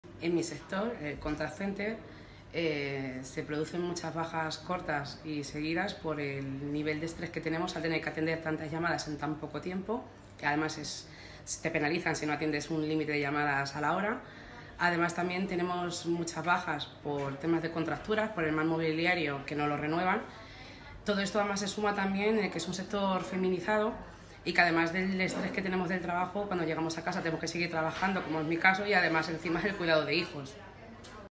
Enfermedades profesionales. Contact Center
Raquel.  Sector del ‘contact center’. En mi sector se producen muchas bajas cortas y seguidas por el nivel de estrés que padecemos, al tener que atender un número de llamadas determinado en poco tiempo. Si no te penalizan.

Además, es un sector feminizado y al acabar nuestra jornada, en casa, tenemos que seguir trabajando en cuidados de familia, lo que repercute en nuestras dolencias”.